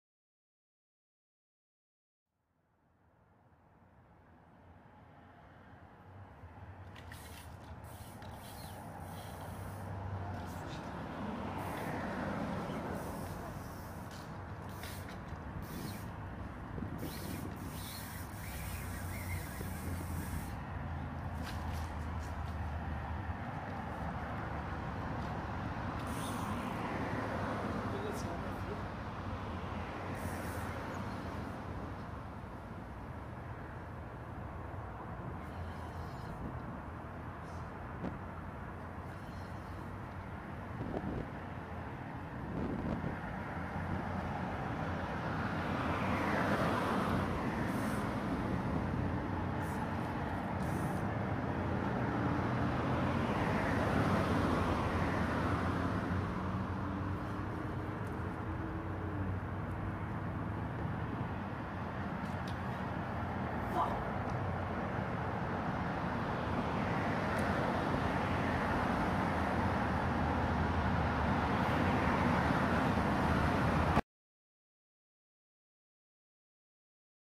Carp Fishing 2010
Location: South Platte River
Fish: Carp
Bait: Macaroni